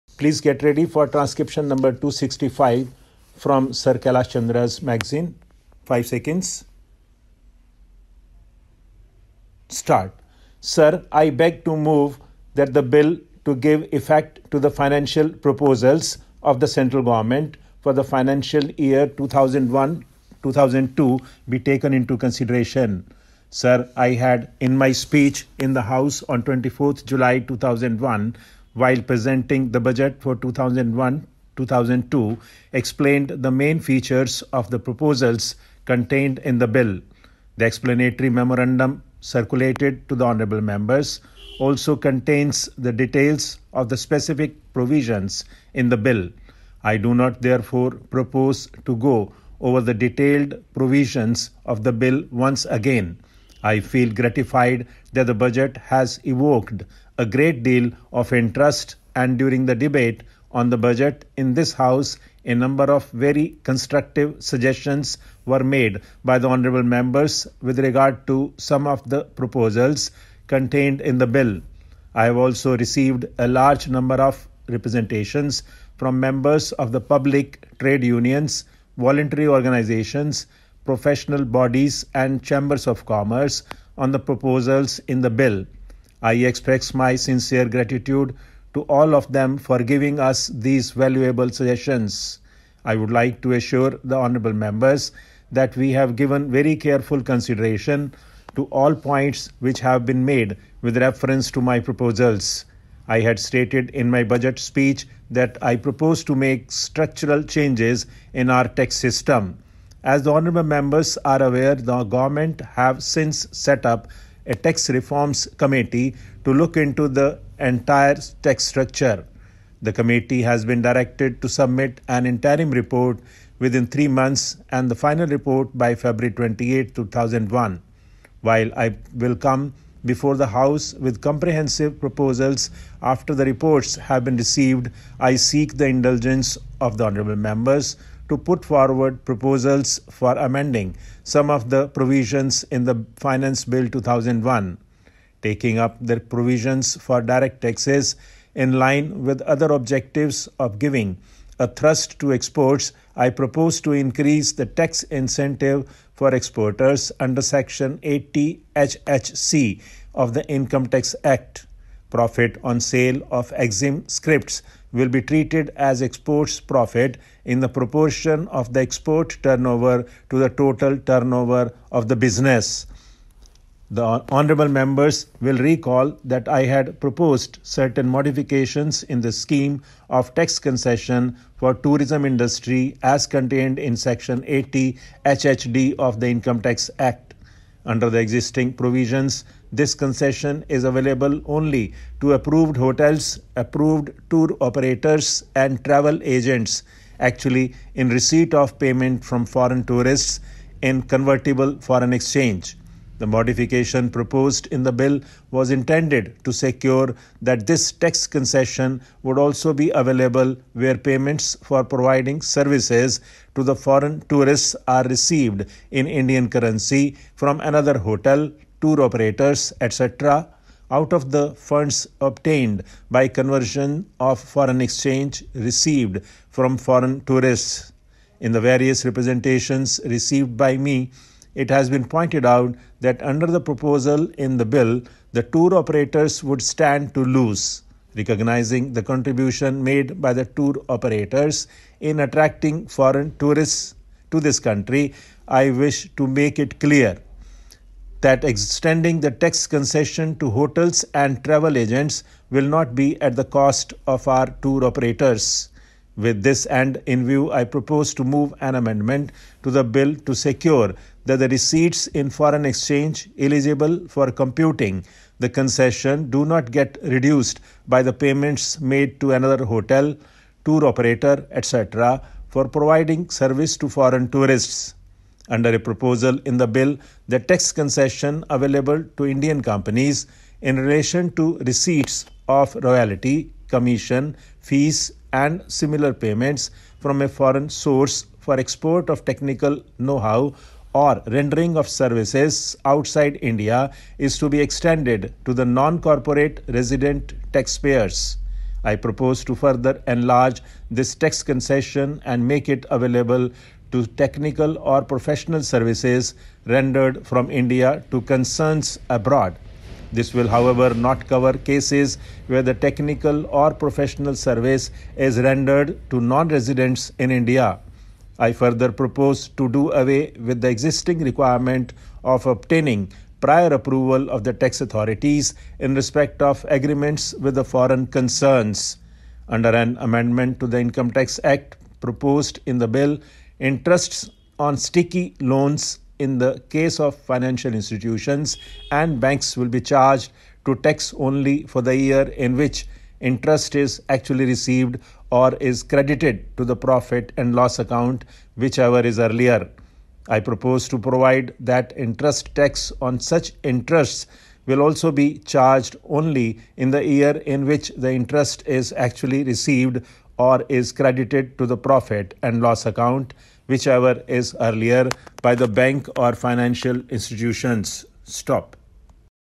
Dictation from Kailash Chandra magazine - Exercise 265 @ 140 wpm
Hello friends,
This video contains an audio dictation of above 1 exercise @ 140 wpm for 6 mins – 840 words.
The link of the passage is given below:-

I would suggest you to take out an hour’s time exclusively to do the transcription
THANKS